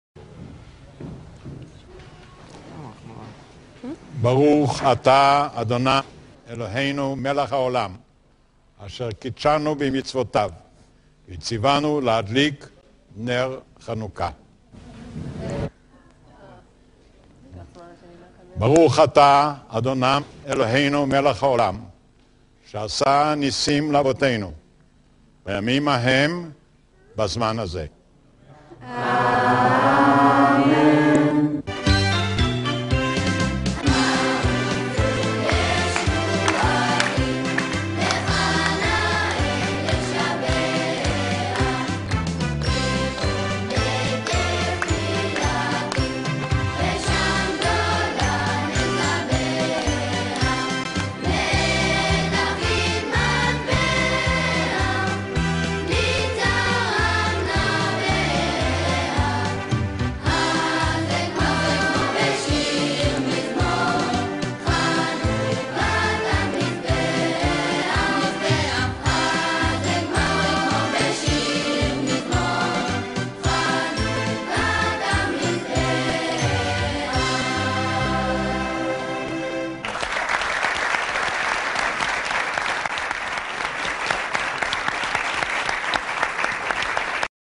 מקהלת פרחי פתח תקוה -בבית הנשיא - מנצח: אבי ברק
מקהלת פרחי פתח תקוה- פועלת בחסות האגף לתרבות תורנית בעיריית פתח תקוה. בראשותו של משה פלדמר ובניהולו של שמוליק זלצר.